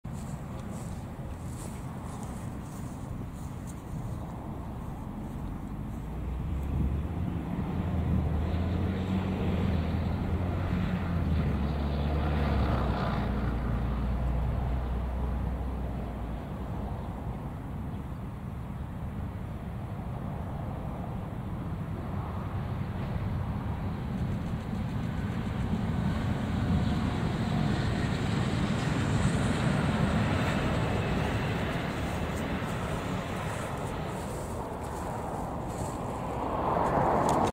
ETR 421 treno 11 San Giorgio Di Piano
Regionale da Ferrara a Bologna Centrale affidato al rock a 4 elementi treno 11, in transito poco prima di San Giorgio di Piano.